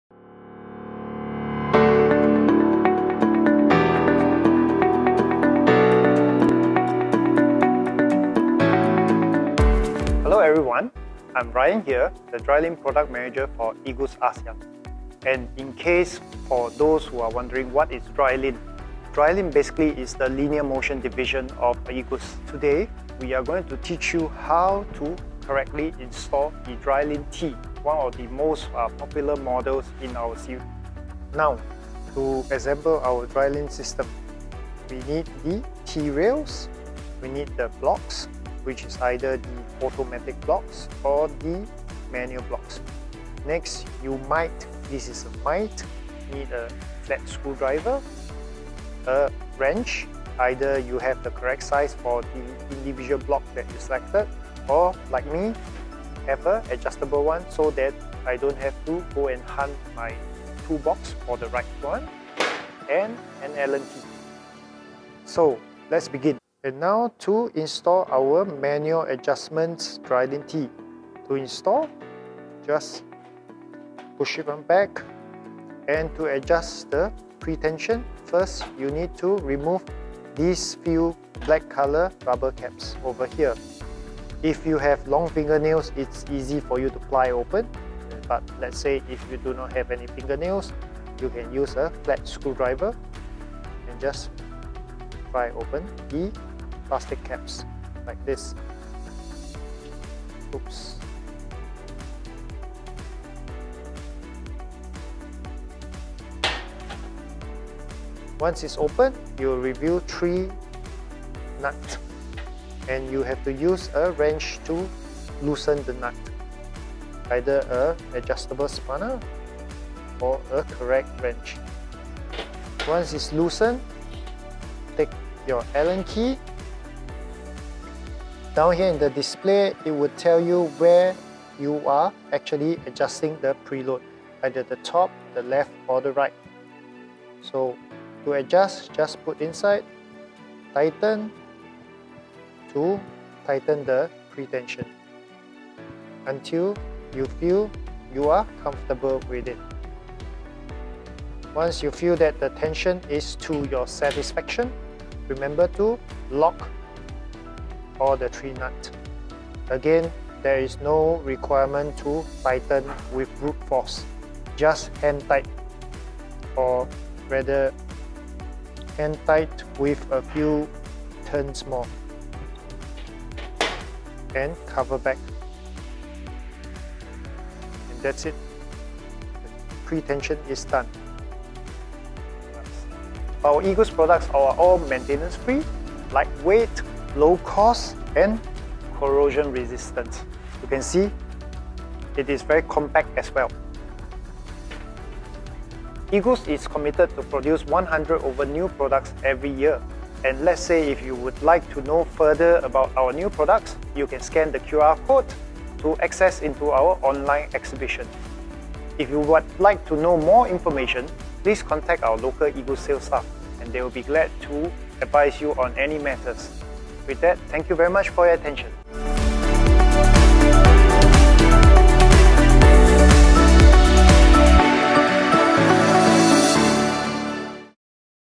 How to assemble drylin® T (Manual adjustment)
igus® linear guide systems are based on sliding instead of rolling. Additionally, they are robust, lubrication-free, corrosion-free, dirt-resistant, lightweight, and quiet.